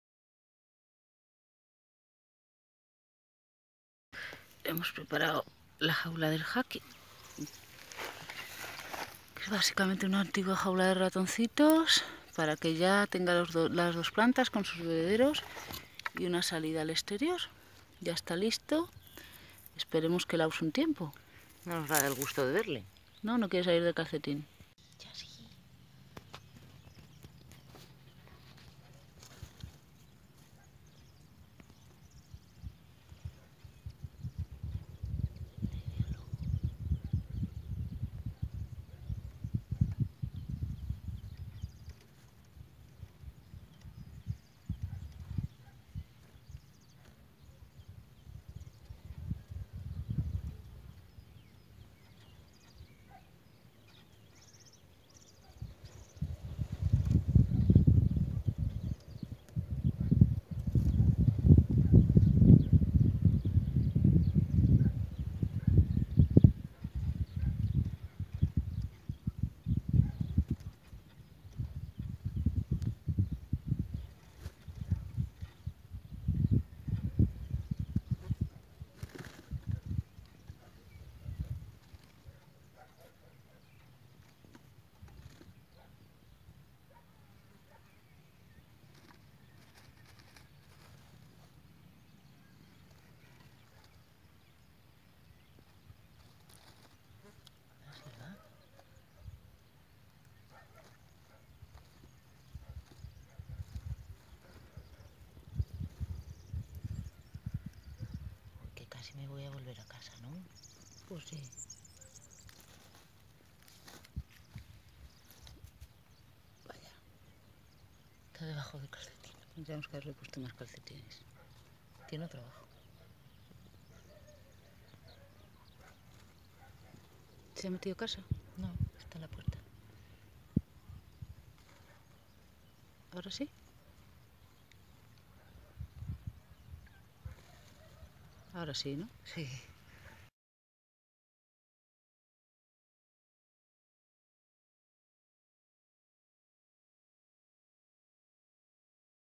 Hacking Sylva (Apodemus sylvaticus)
Os acordáis del ratón de campo lactante?
ya ha crecido y hemos preparado su hacking para su liberación.
Esperamos que le vaya muy bien!!
es precioso y ha sido una experiencia maravillosa tenerlo este tiempo en el santuario